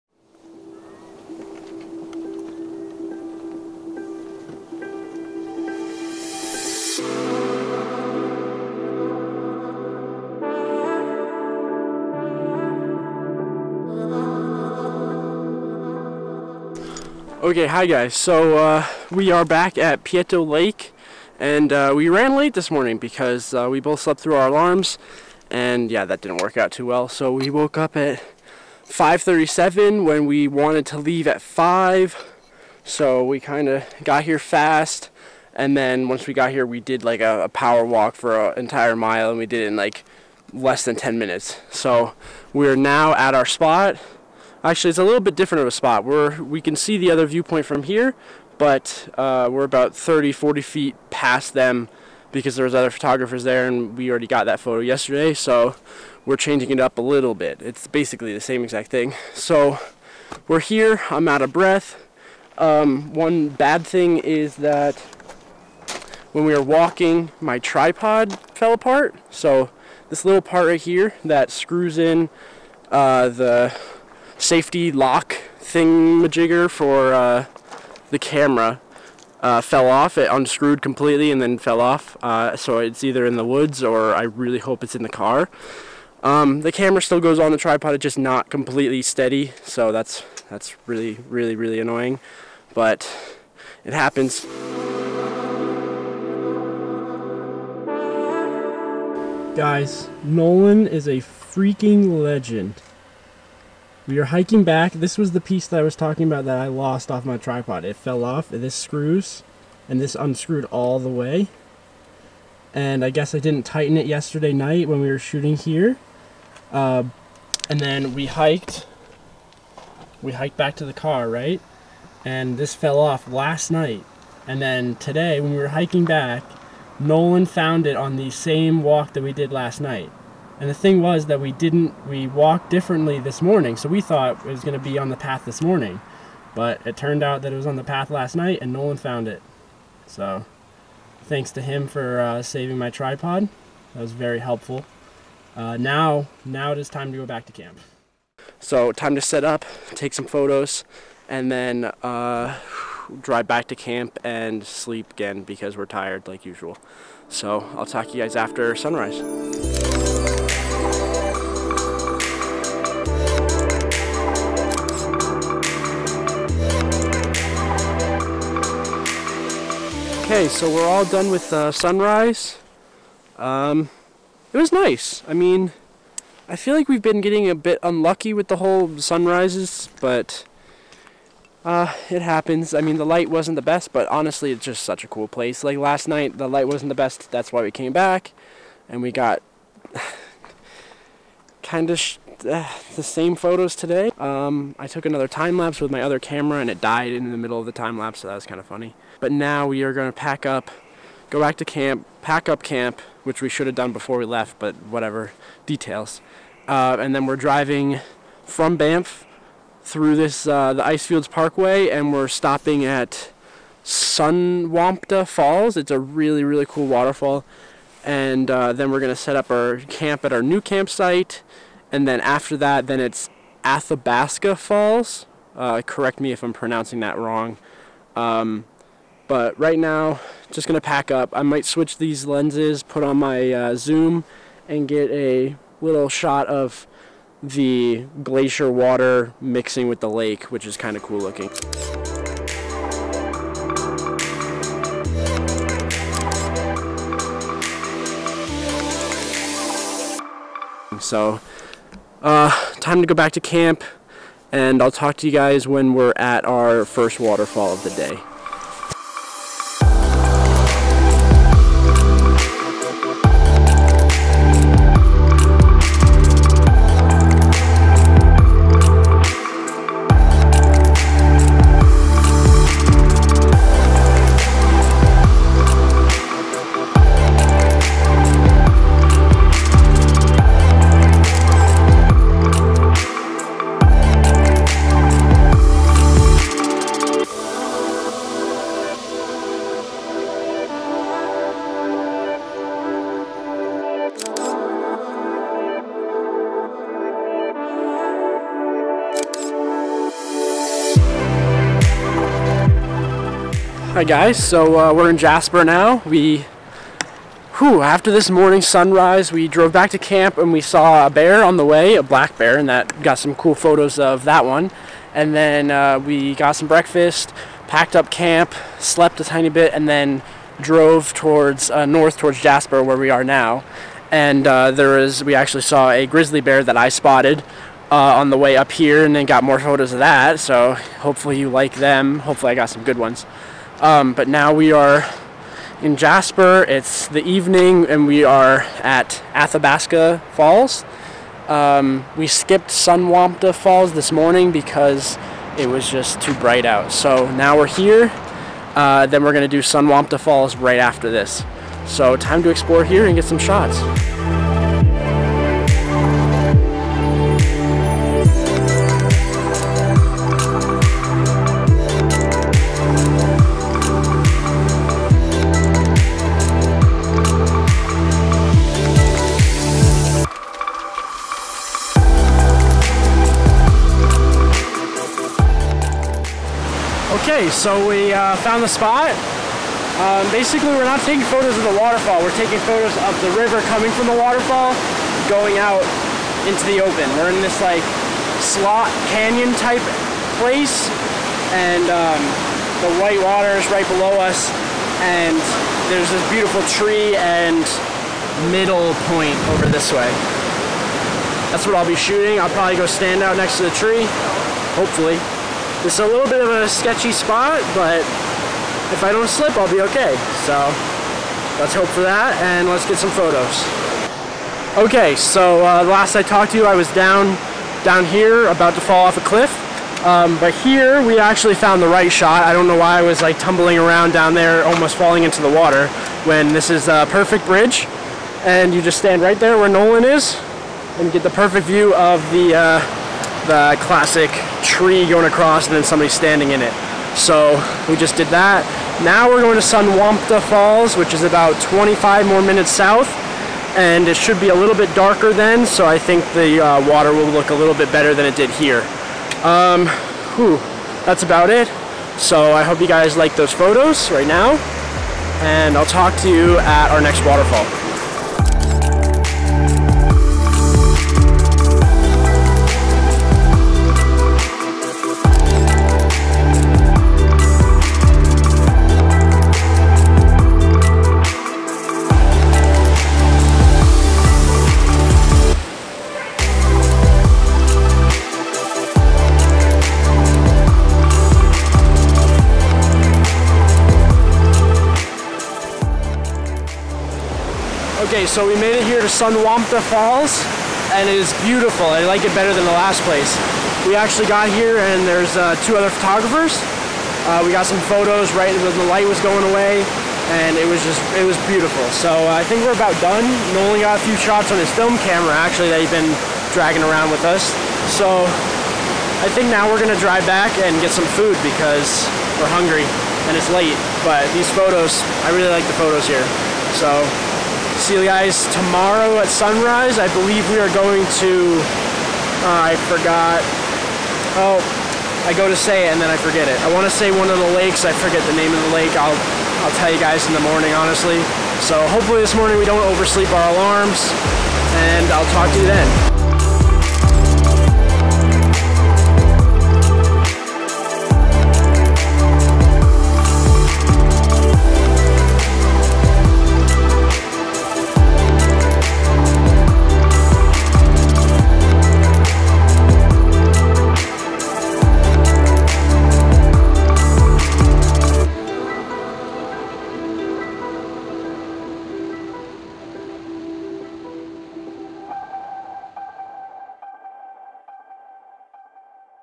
Waterfalls in JASPER NATIONAL PARK, CANADA - Road Trip Photography Vlog #6
ROAD TRIP VLOG #6

Welcome to the sixth video from this past summer's road trip with Nolan and I!

In this episode we start in northern Banff National Park at Peyto lake and make our way further north up the icefields parkway into Jasper National Park. We see some epic wildlife and waterfalls along the way!

Photo Locations:

Sunrise - Peyto Lake
Midday - Athabasca Falls (but really the cliffs past the waterfall)
Sunset - Sunwapta Falls

I hope you enjoy and as always I'll talk to you guys in the next video :)